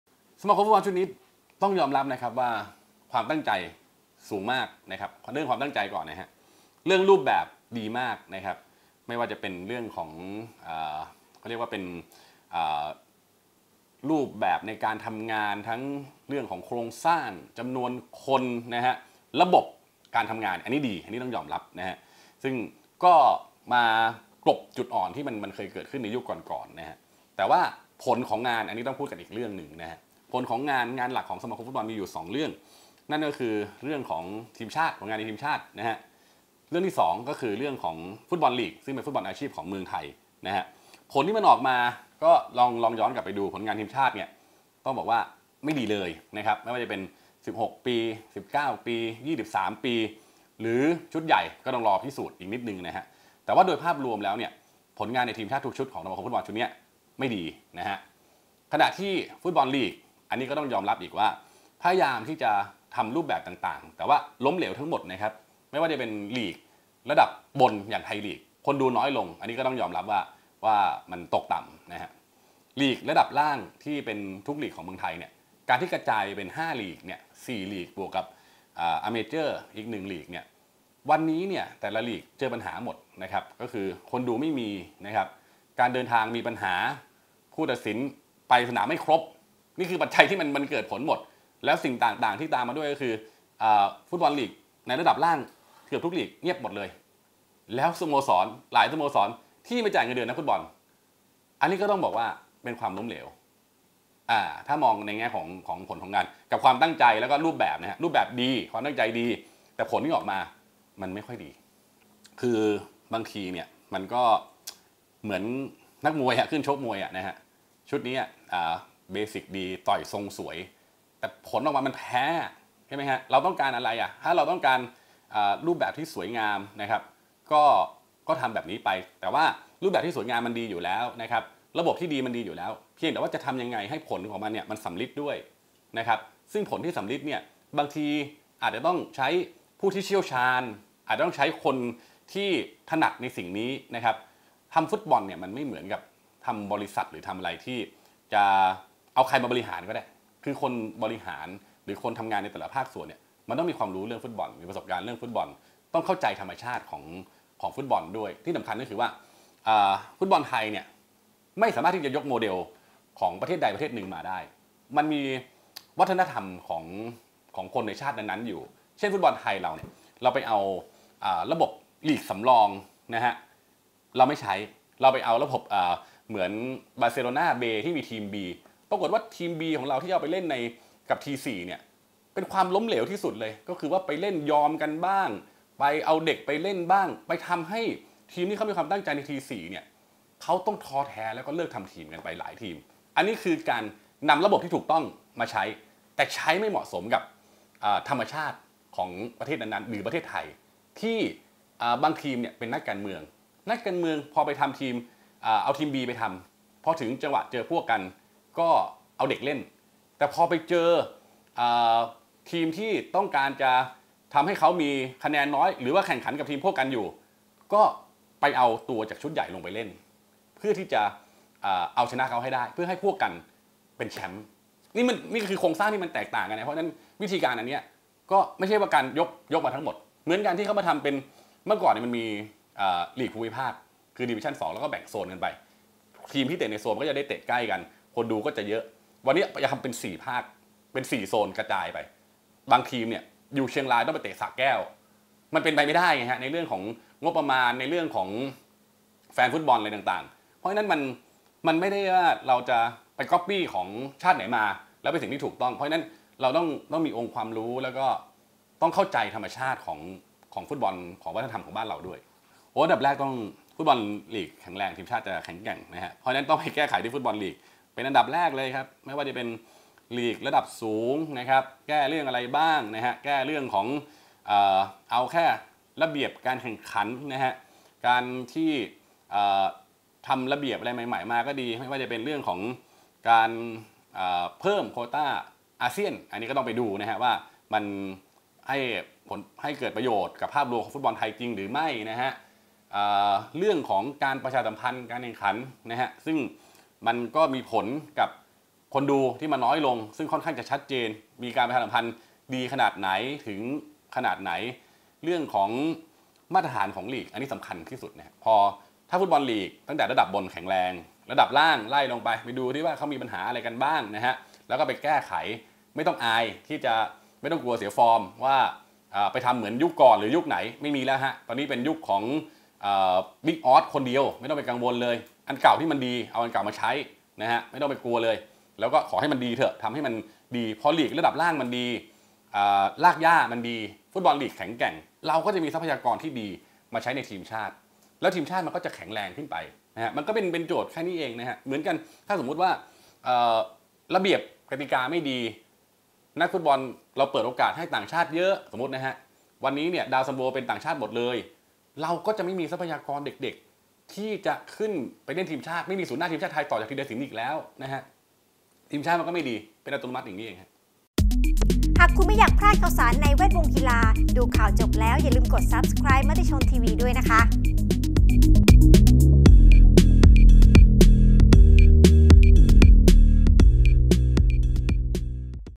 ปูเป้-สมฤกษ์ สะท้อนการทำงานสมาคมกีฬาฟุตบอลฯ ยุคบิ๊กอ๊อด
Official Matichon TV "ปูเป้" สมฤกษ์ อิศรางกูร ณ อยุธยา คอลัมนิสต์ นสพ.สยามกีฬา พูดถึงการบริหารงานสมาคมกีฬาฟุตบอลแห่งประเทศไทยฯ ยุค พล.ต.อ.สมยศ พุ่มพันธุ์ม่วง หลังจากเข้ามารับตำแหน่งนายกสมาคมกีฬาฟุตบอลฯ เมื่อเดือนกุมภาพันธ์ 2559

วงการฟุตบอลไทยเกิดอะไรขึ้นบ้าง และทิศทางของฟุตบอลไทยหลังจากนี้จะเป็นอย่างไร ไปฟังคำตอบได้เลย